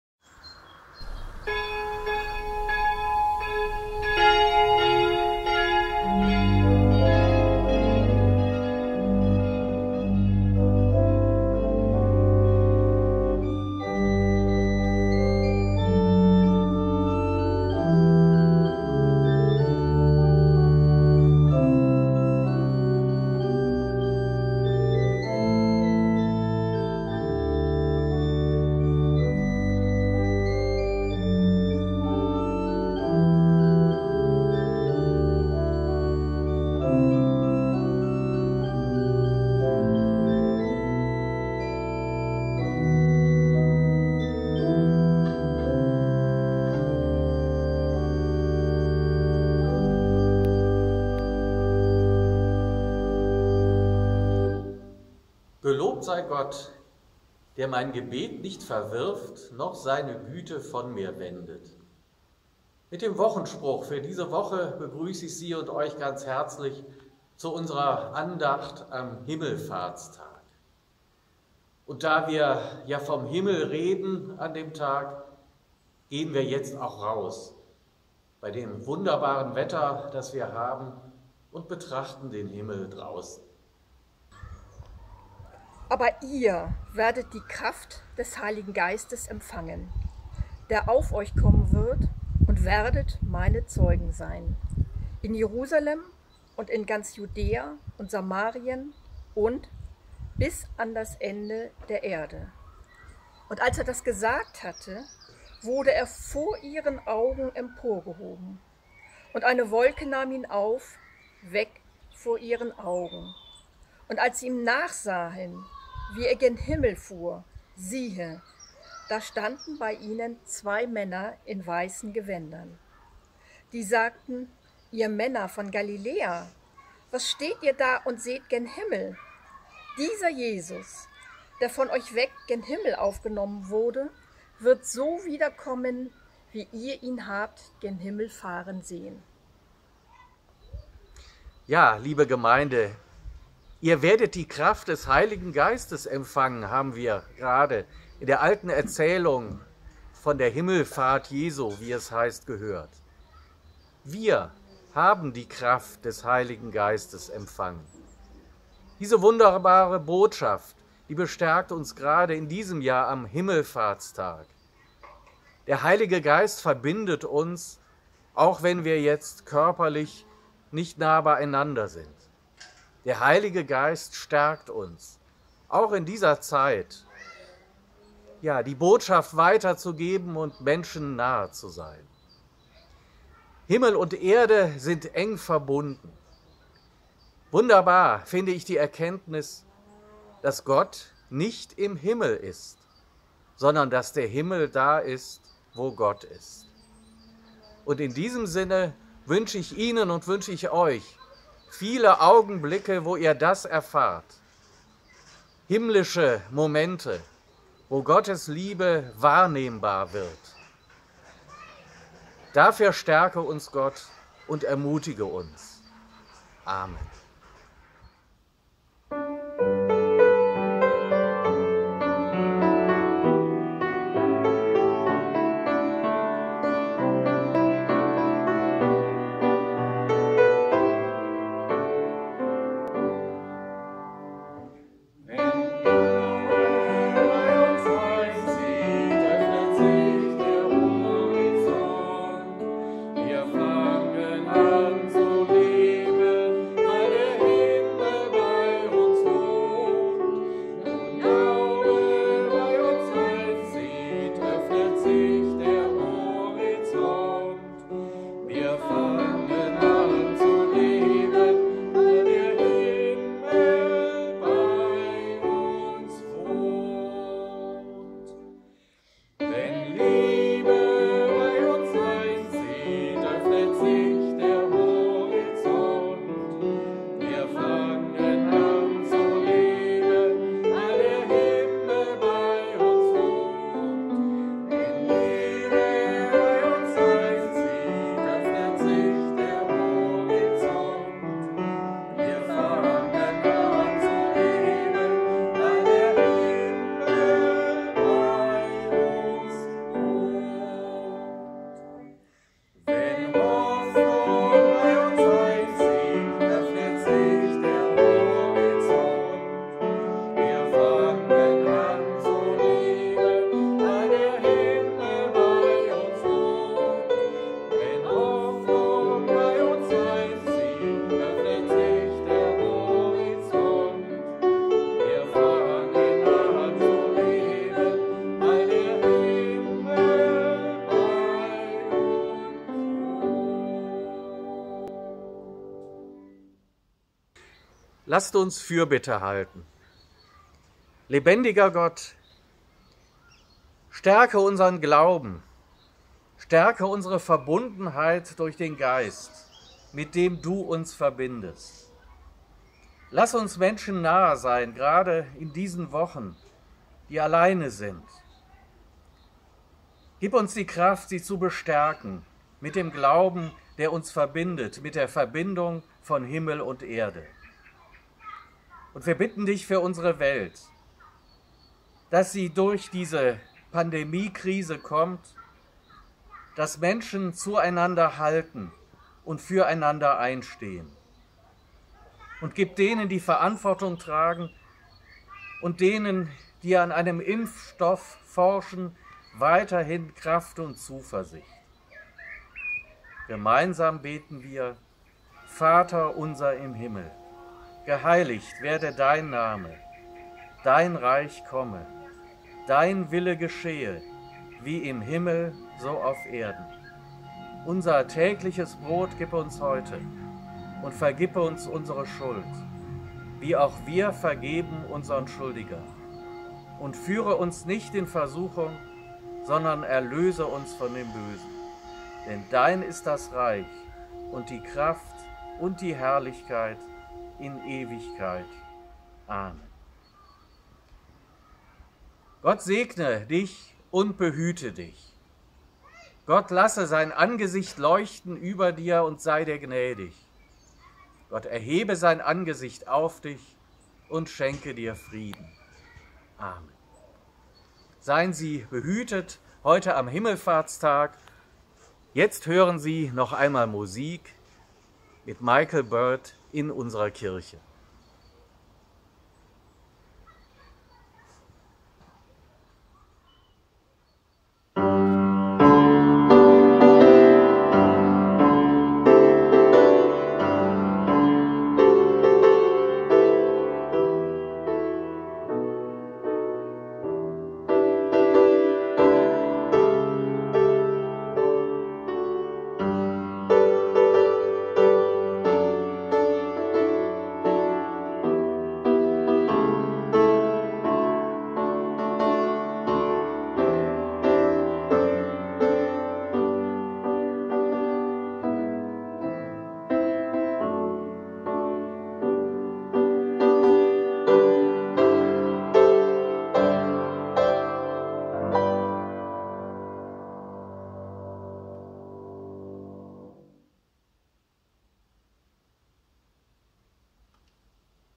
2020 05 21 Christi Himmelfahrt Andacht mit Superintendent Markus Zimmermann
Andacht zu Christi Himmelfahrt mit Superintendent Markus Zimmermann